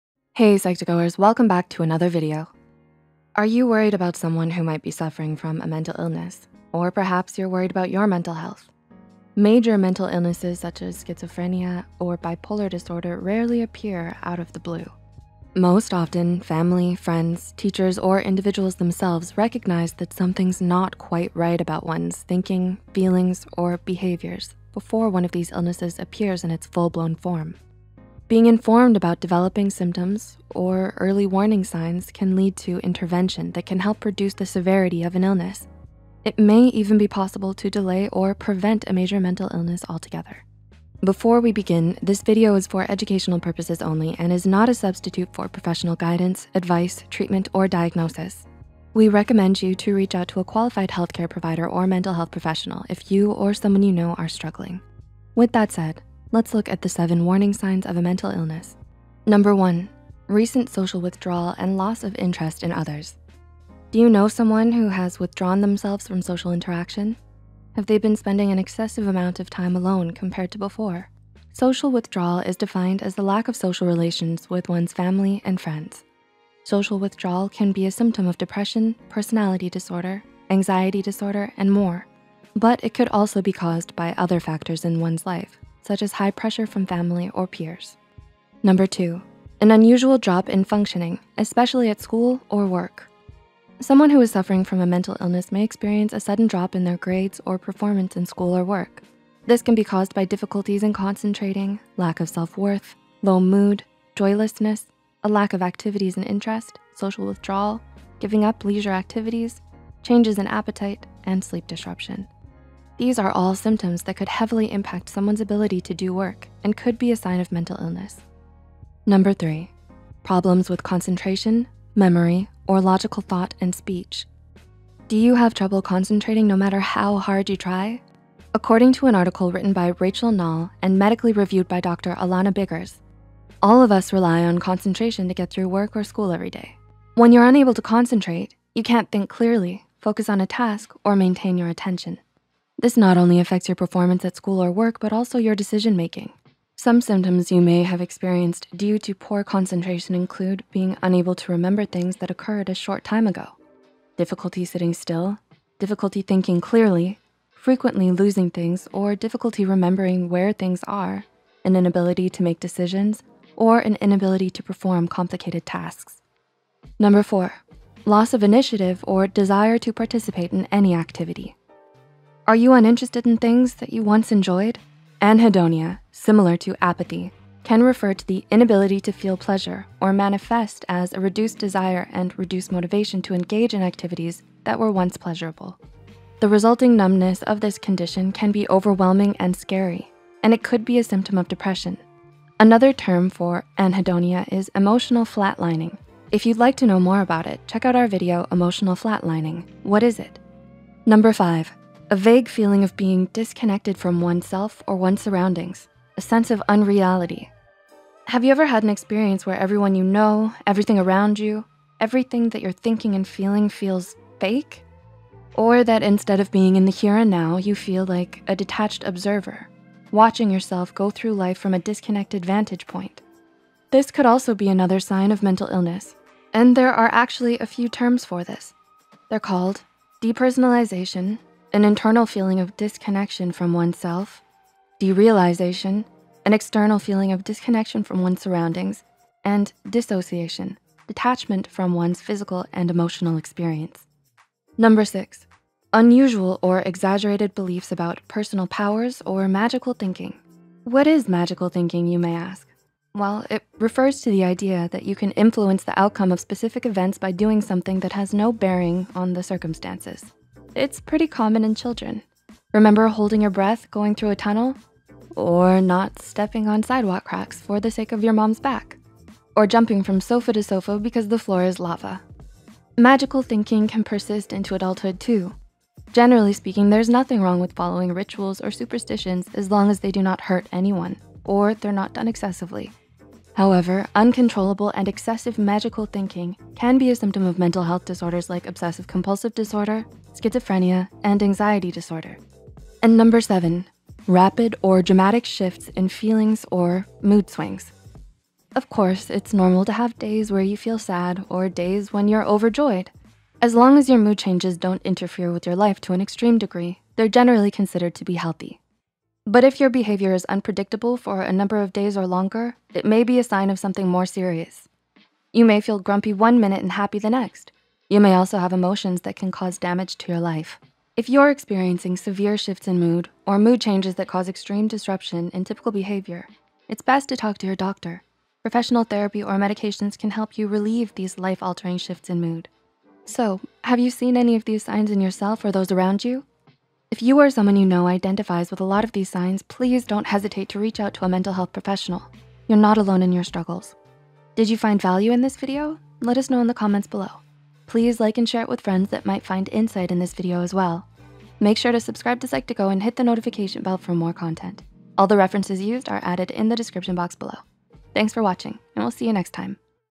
7 Warning Signs Of A Mental Illness
Are you worried about someone who might be suffering from a mental illness? Or perhaps you’re worried about your mental health? Major mental illnesses such as schizophrenia or bipolar disorder rarely appear “out of the blue.” Most often people recognize that something’s not quite right about one’s thinking, feelings, or behavior before one of these illnesses appears in its full-blown form. Being informed about developing symptoms, or early warning signs can lead to intervention that can help reduce the severity of an illness. It may even be possible to delay or prevent a major mental illness altogether. So to help you with that, here are a few common warning signs of a mental illness.

Writer: Syazwana Amirah
Script Editor: Vanessa Tao
Script Manager: Kelly Soong